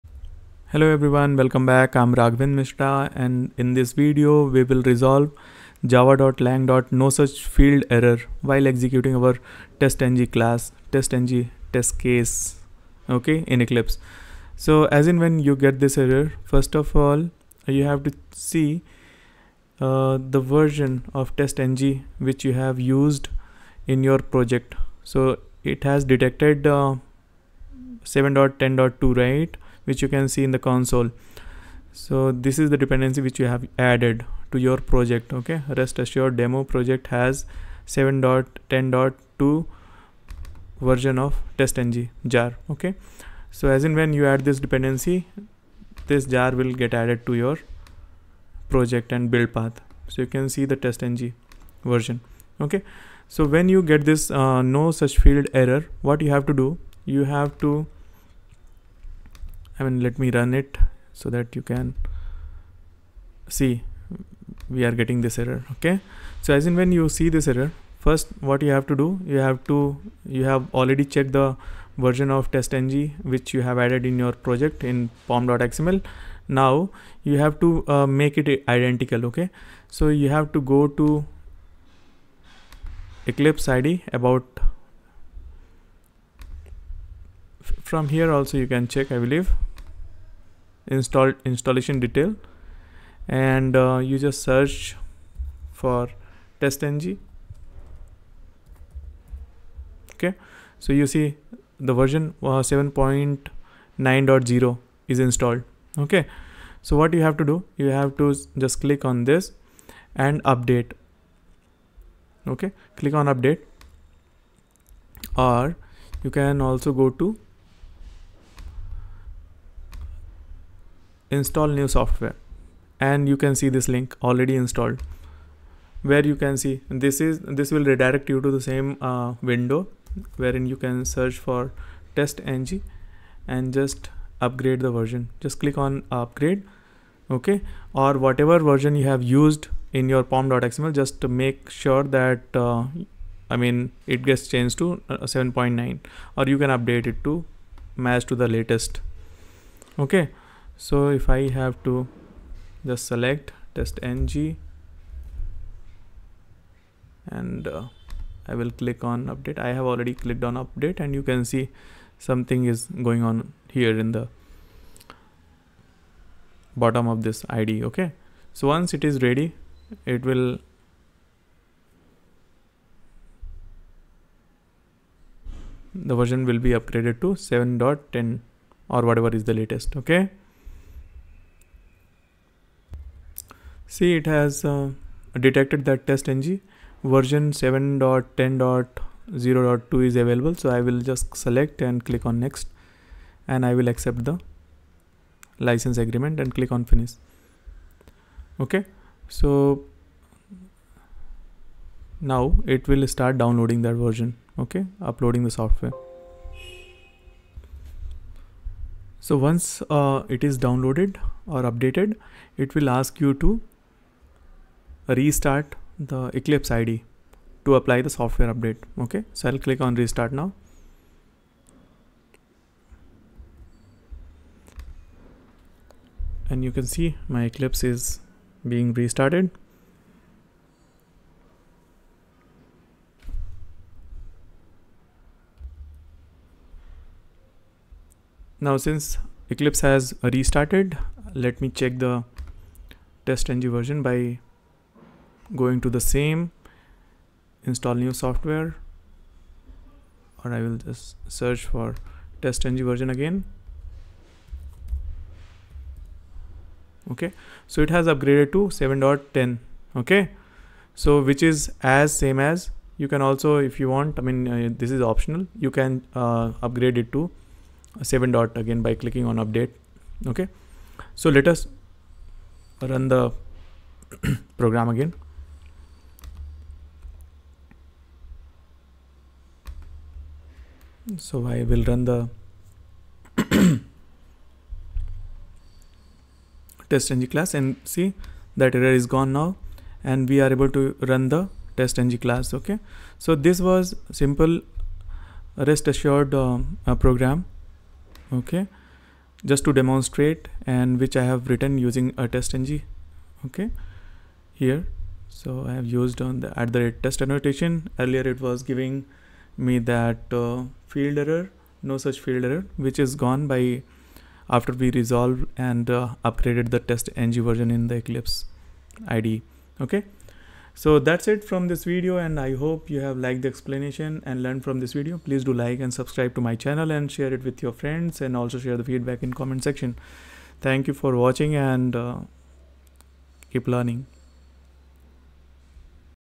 How to fix "Exception in thread "main" java.lang.NoSuchFieldError: port" in TestNG| BDD| RestAssured
There was a problem running a TestNG test case. A "NoSuchFieldError" exception arose in the main thread. This suggests that the code is trying to access a field named "port" that doesn't exist in the relevant class. This could be due to a class version mismatch or a typo in the field name.

In this video, I've shown how to resolve the following error encountered during execution of a TestNG class, whether using Selenium WebDriver or Rest Assured.

import org.testng.annotations.Test;
import io.restassured.RestAssured;
import io.restassured.response.Response;

public class FirstRestAssured {
 public void test1()
 System.out.println(response.statusCode());
 System.out.println(response.getTime());

Console Error:
[RemoteTestNG] detected TestNG version 7.10.2
SLF4J: Failed to load class "org.slf4j.impl.StaticLoggerBinder".
SLF4J: Defaulting to no-operation (NOP) logger implementation
for further details.
Exception in thread "main" java.lang.NoSuchFieldError: port
 at org.testng.remote.RemoteTestNG.main(RemoteTestNG.java:69)

Selenium Grid Tutorial For Beginners [2024 latest] - End to end - By SDET Adda for QA Automation